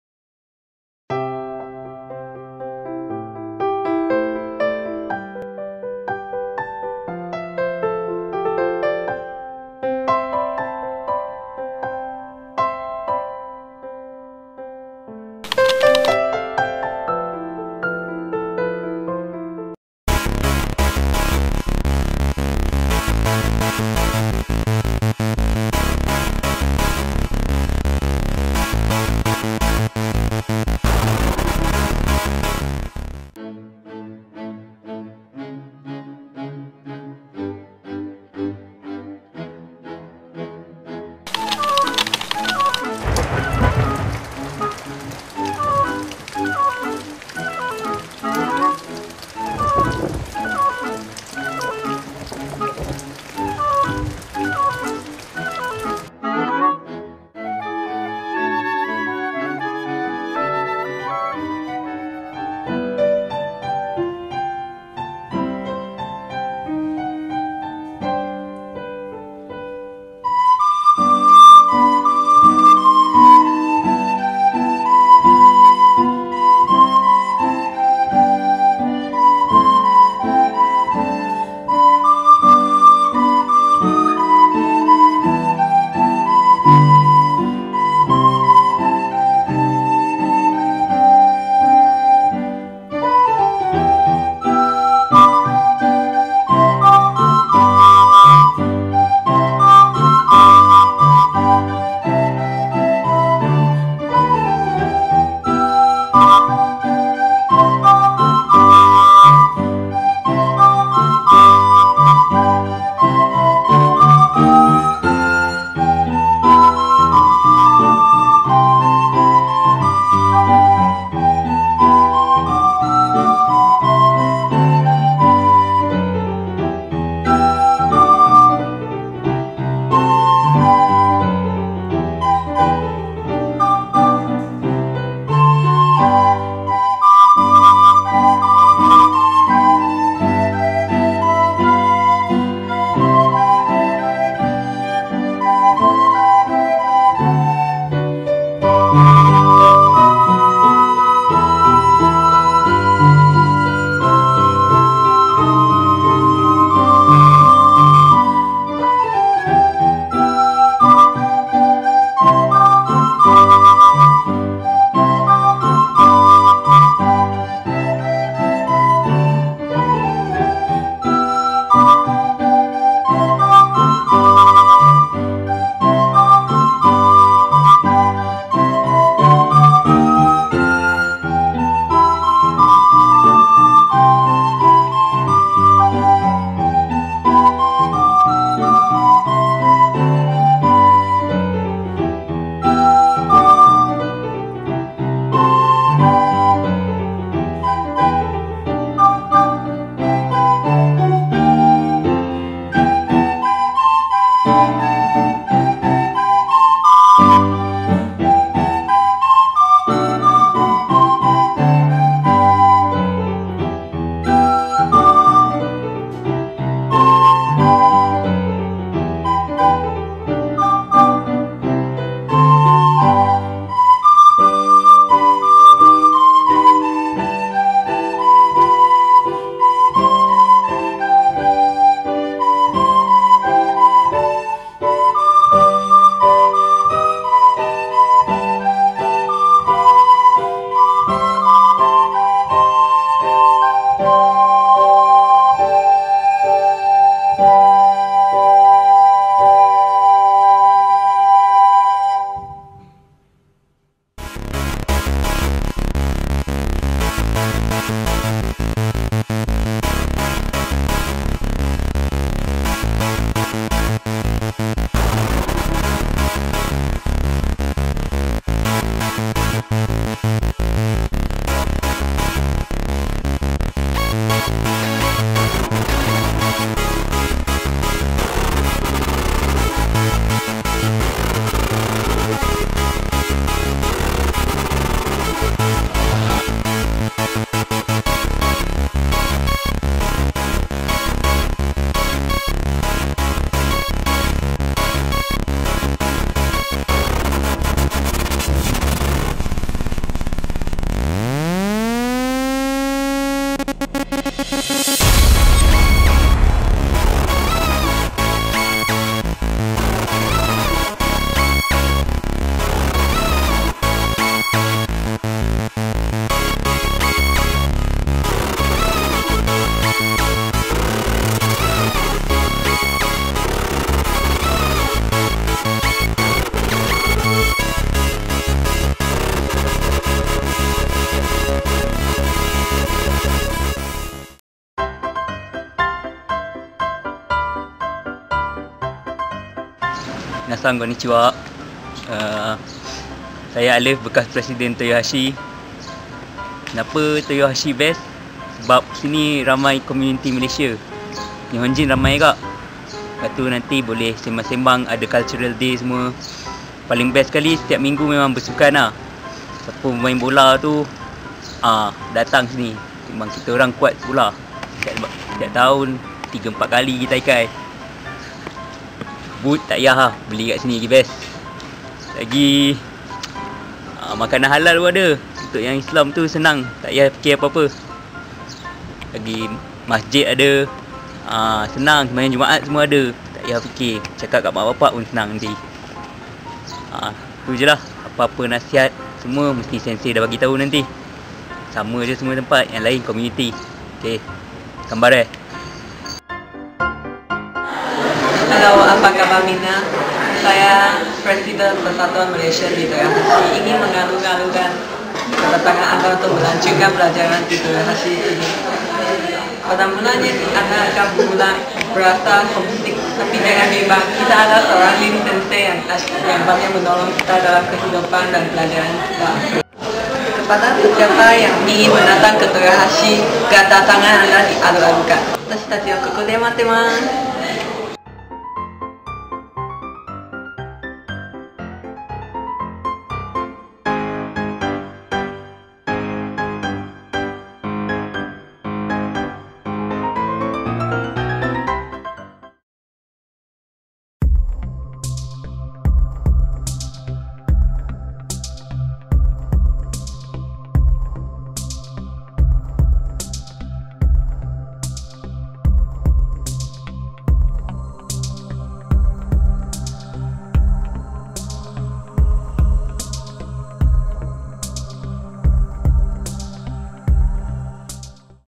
VIDEO MESSAGE for JUNIOR [2013]
A video message which shows our life here in Toyohashi,Japan for juniors who are coming. ^^